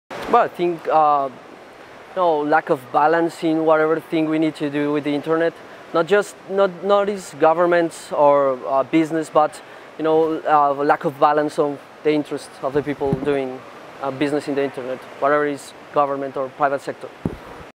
Global IGF 2015: Juan Diego Castañeda Gomez on Biggest Net Threat
Juan Diego Castañeda Gomez, researcher at the Karisma Foundation, shares his answer to the question, “Experts say one of the biggest threats to the Internet is the ways in which businesses and governments seek to control people’s interactions online. What do you see as the greatest threat to achieving the most positive future for the global Internet?” Recorded at the Global Internet Governance Forum 2015 in João Pessoa, Brazil, by the Imagining the Internet Center, an initiative of Elon University, North Carolina, USA.